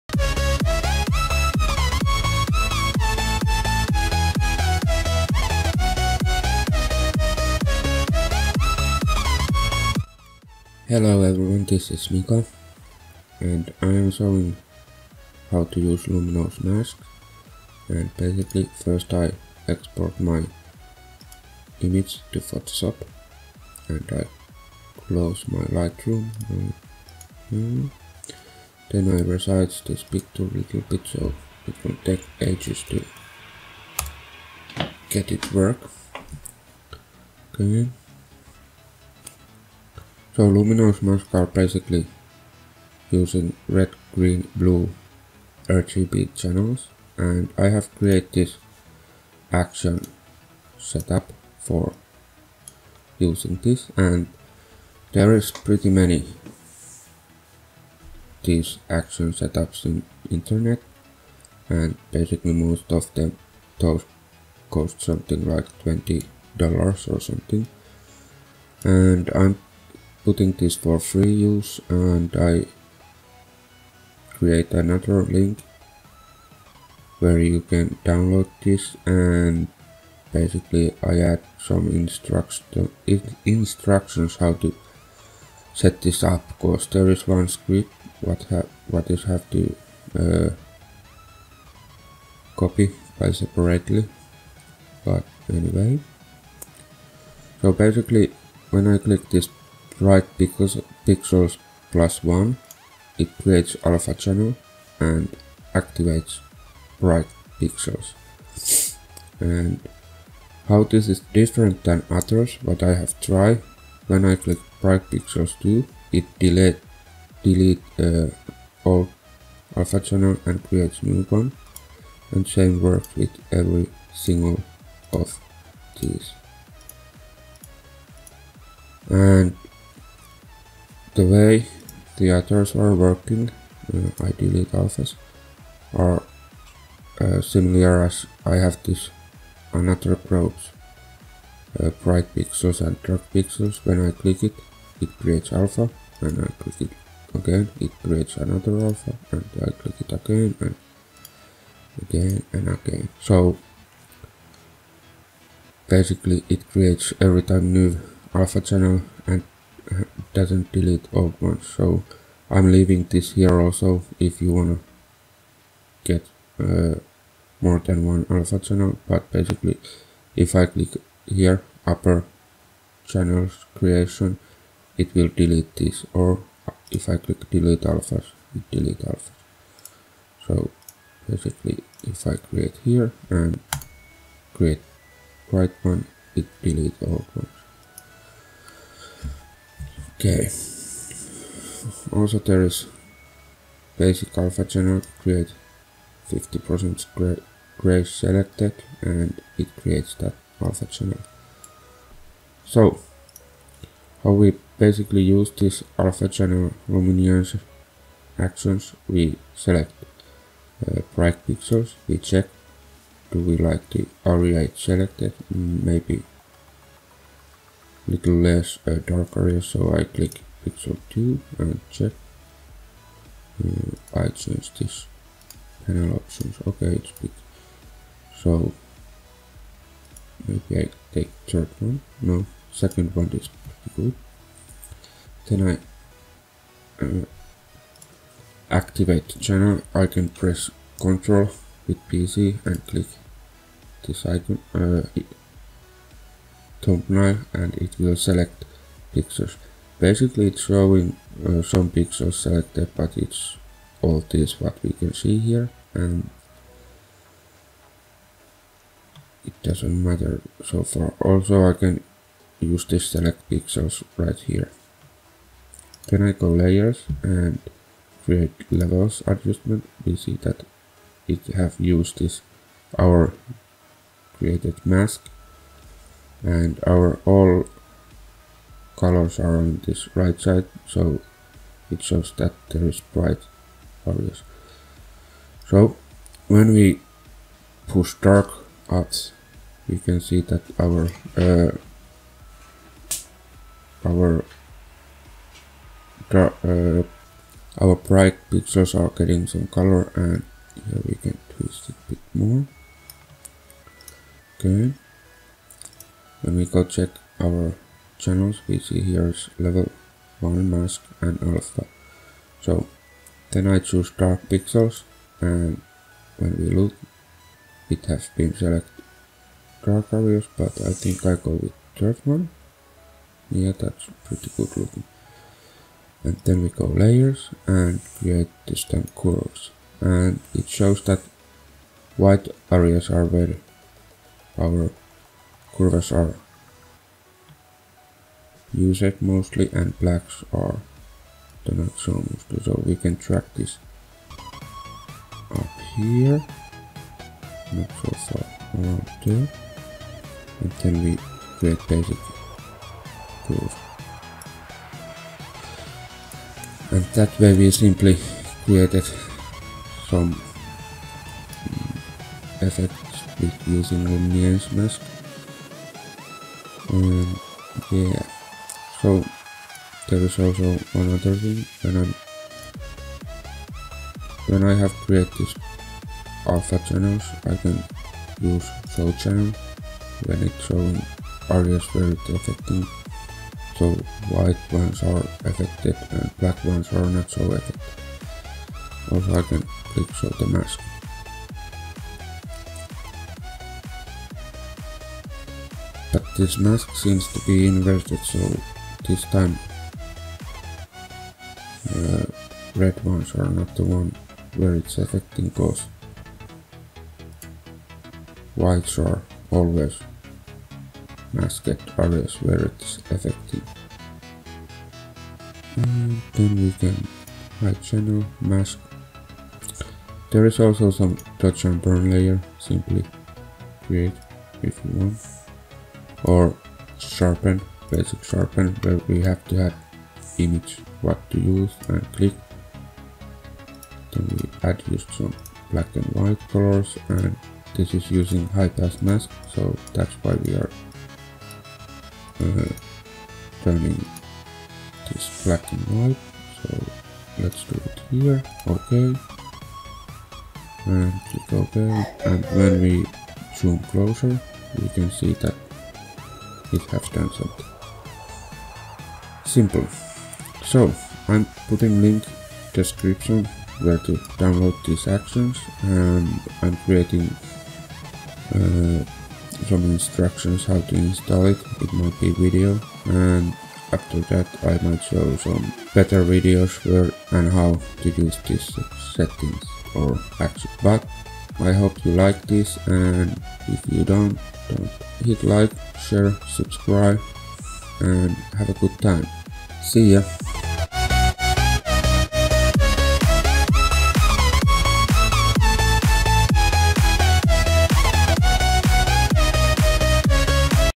#2 What does everything do. more or less.
Here in this video i will show more closely what every actions basically does, and also few example how to use it.
Next video will be more like real usage of this action tool.